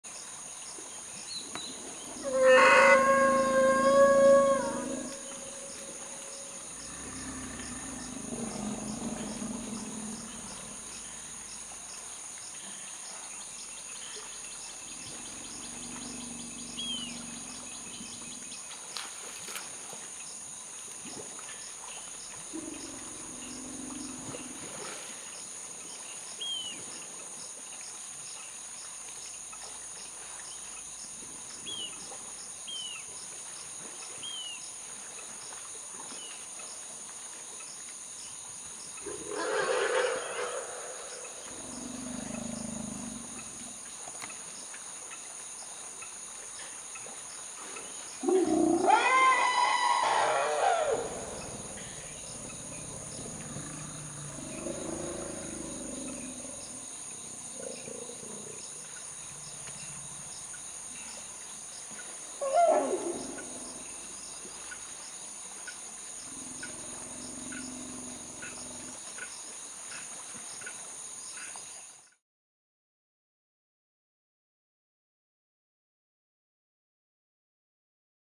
Young female elephants care for a lost calf
Here, an infant male calf who appears to be lost, gives off a loud high pitched scream. Two young females approach him and vie with each other for the privilege of caring for and allomothering him. The infant’s mother then approaches the two females and drives them away from him.